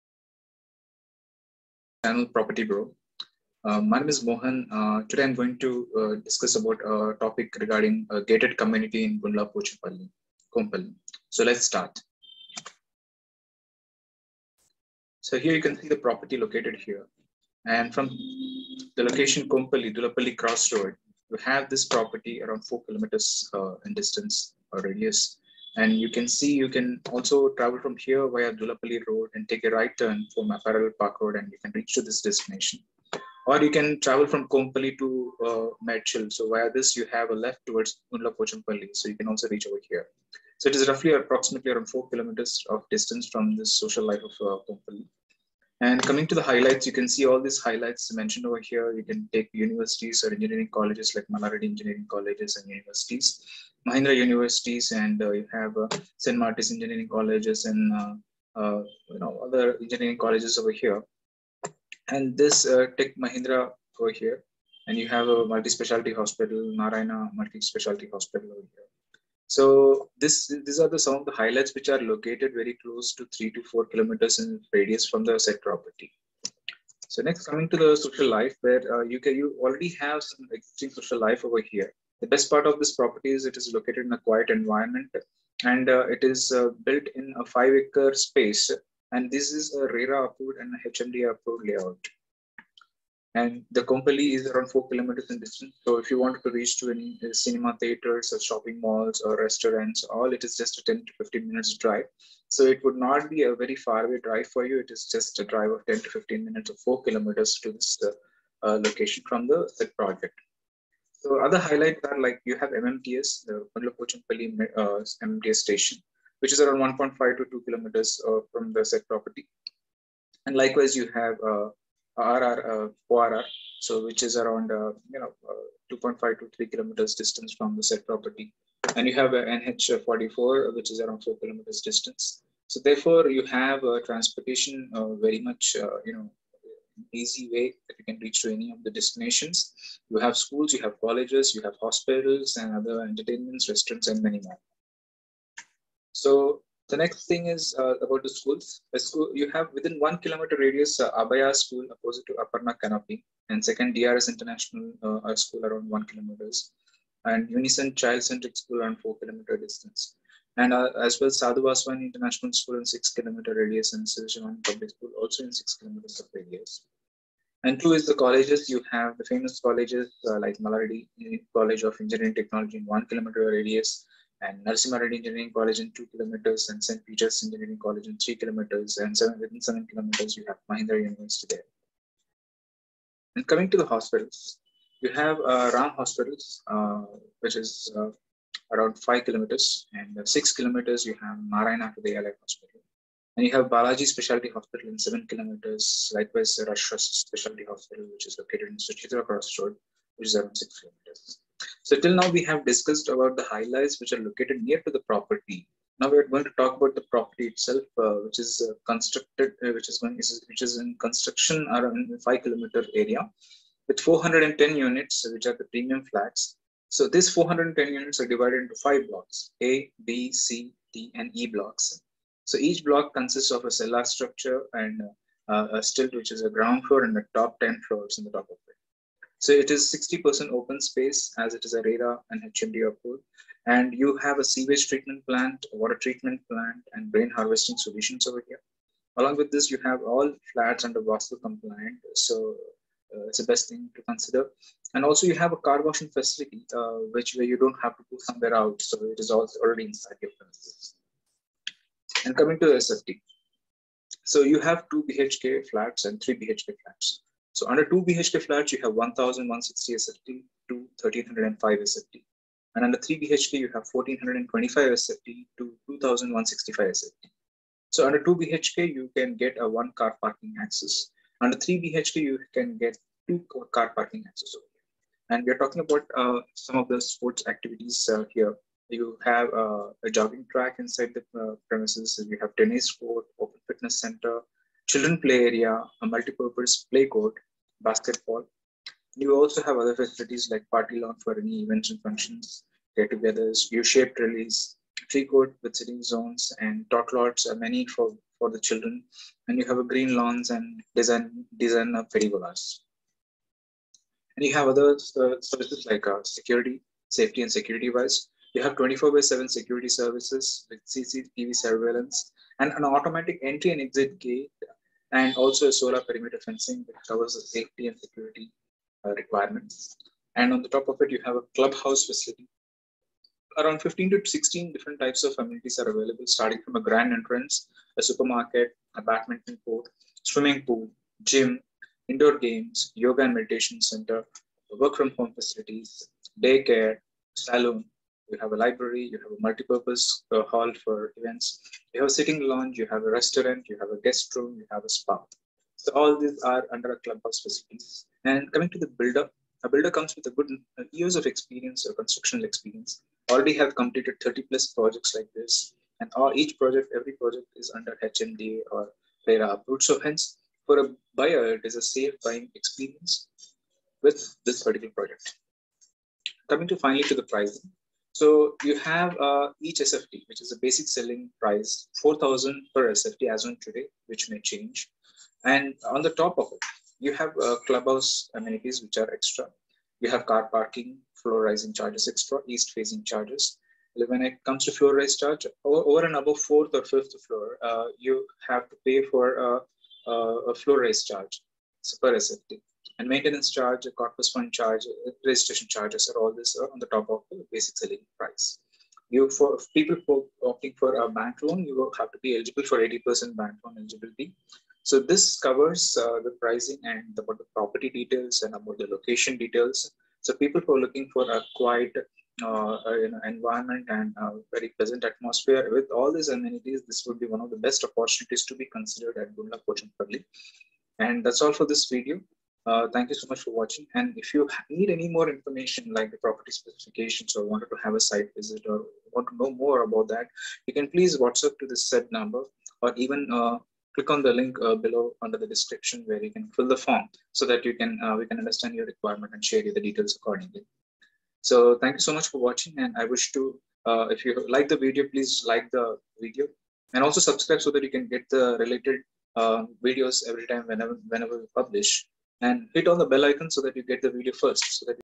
Gated Community in Gundla Pochampally - Kompally - English Detailing
Hello ! I'm  from Propertybro ..a trained real estate consultant.
my objective is to help the buyer with best & safe option available in the market meeting your dream homes.

Today the topic is about a Gated Community in Gundla Pochampally location which is close to kompally.

This Property is located close the local & Social life like Transporatation,Schools & Colleges, Hospitals,Entertainment,Restaurants, and many more.

In Transportation, this property is close to MMTS by 1.5 km range appx. and ORR by 2.5 km. which helps to reach airport or any other destination from the said property
without any traffic challenges.and NH-7 around 4 kms.

About Hospitals - Around 5kms to Ram Hospitals.
                - Around 6kms to Narayana Hridayalaya .
  - Around 7kms to Balaji Speciality Hospital.

About Schools - Abhaya School Opp to Aparna Kanopy.
              - DRS International around 1 kms.
       - UNICENT Child centric school - 4kms.
              - Sadhu Vaswani International School -6 kms.
              - Siva Sivani Public School -6 kms.

Near Reputed Colleges  - Like Malla reddy College of Eng. Technology is 1kms.
          - Narsimha Reddy Engineering College - 2kms.
         - St.Peters Engineering College - 3kms.
         - Mahindra University -7 kms.

Entertainment & Restaurants - Property is close to Kompally by 4-5 km to meet all your requirements.

ABOUT PROPERTY - This 5 Acre luxurious Gated Community is built in 5 Acre area with 5 independent blocks each like A,B,C,D & E with cellar + Stilt +10 Floors.
               - This property is an approved RERA Site with 60% left to open space with 410 apartment units and having apartment size range
                 from 2BHK : 1160 SFT -1305 SFT. & 3BHK :1425 SFT- 2165 SFT.
        - An HMDA Approved Project.
               - Vaastu Complaint Units & 2 Levels of car parking space.
        - Rain Harvesting Solutions.
        - Car Washing Facility.

OUTDOOR SPORTS & ACTIVITY : 1. Basket Ball 2. Multi Purpose Play Court 3. Open Fitness Center 4.Tennis Court 5. Jogging Track 6.Children Play area.
OTHER FACILITIES - Party Lawn 2. U-SHAPED Trellis 3. Tree Court With Sitting Zones 4. Ample TOT-LOTS 5.Lush Green Lawns.6. Designer Perigolas.
SAFETY & SECURITY - 1.24/7 Security Services. 2. CCTV Surveillance 3. Solar Perimeter Fencing. 4. Automatic Entry & Exit System.

CLUB HOUSE AMENITIES :

1.Grand Entrance Lounge
2.Supermarket
3.Badminton Court
4.Swimming Pool
5.Gym
6.Indoor Games Zone
7.Yoga / Meditation Room.
8.WFH Facility
9.Day Care
10.Salon
11.Library
12.Multipurpose Hall
13.Sitting Lounges.
14.Restaurant
15.Guest Rooms
16.Spa

ABOUT THE BUILDER - Builder come with years of experience in constructions field having successfully completed 30+ projects and also the project is also under
RERA approved so as i buyer you can have transparent experience and a safety buying for dream home for you and your family.

PRICING : - As on today , The cost per sft is 3900/- + amenities +floor rise charge + east facing + maintenance charges for 2 years +corpus fund are additional charges.

Bank Loan : For people looking for loans , your profile needs to be eligible for 80% loan. and rest 20% needs to paid the yourself.

and for more information on specification or site visit please fill the form below in the description.

and thats it for this video.. and if you like the video , please give a like, and click on subcribe button and bell icon for more latest information on your dream home.